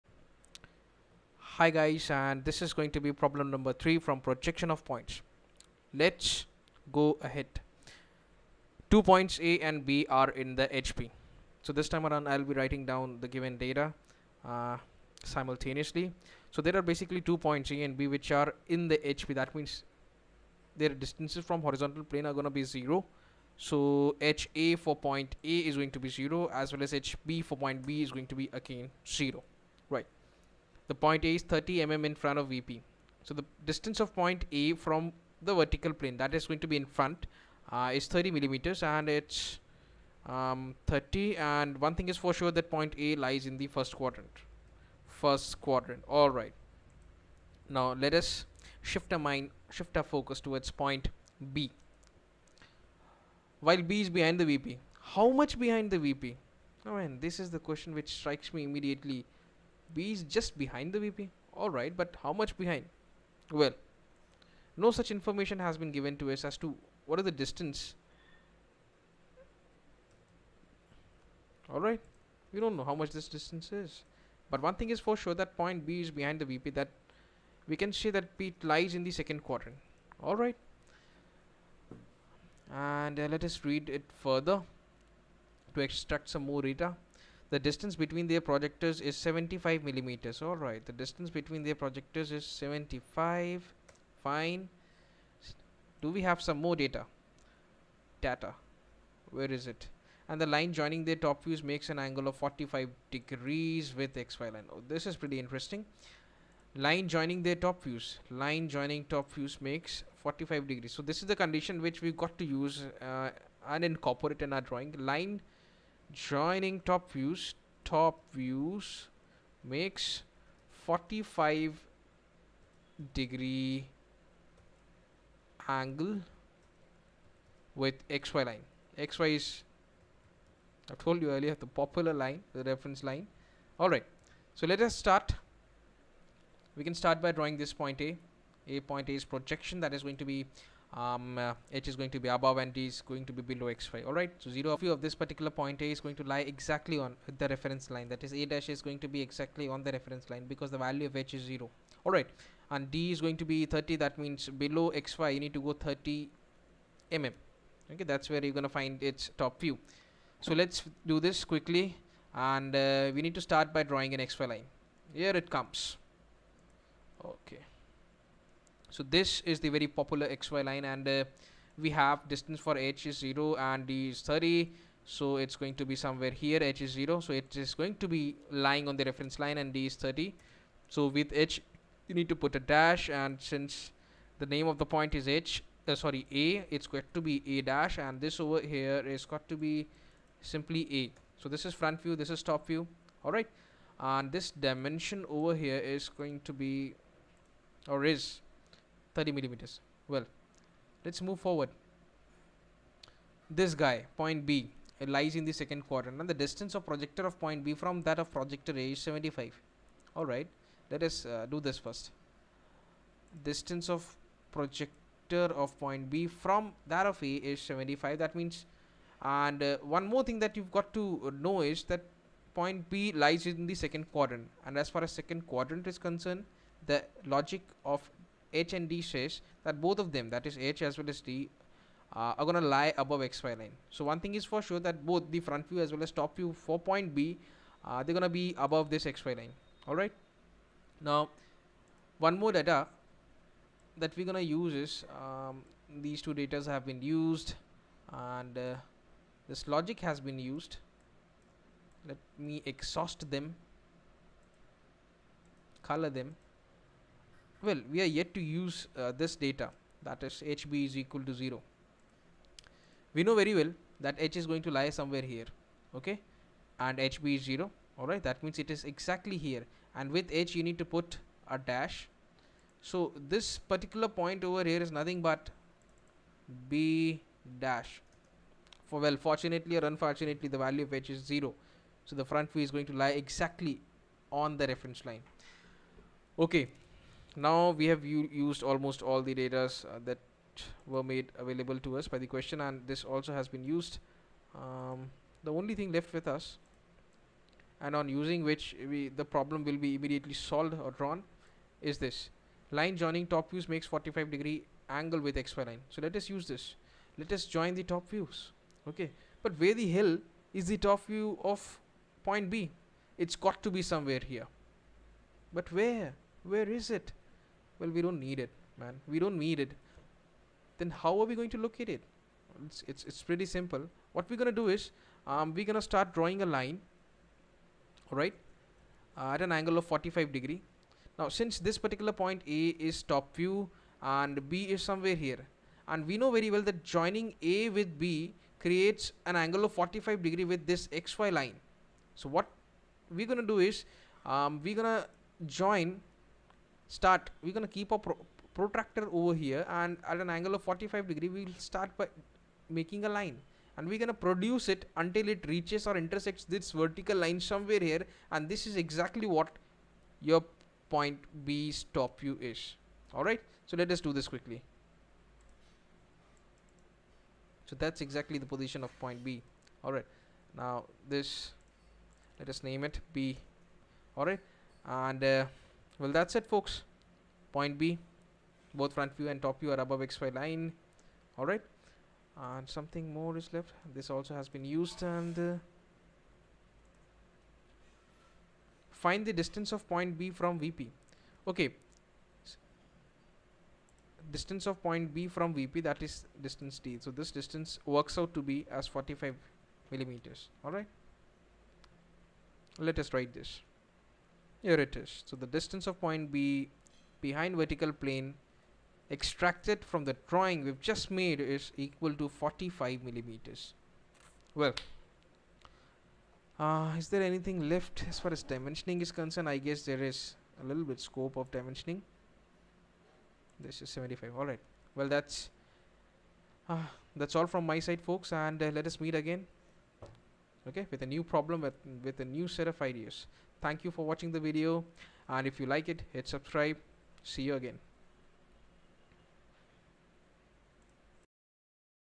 Projection of Points Problem 3
Two points A & B are in the HP. The point A is 30 mm infront of the VP, while B is behind the VP. The distance between their projectors is 75 mm and the line joining their top views makes an angle of 45o with xy. Find the distance of point B from VP.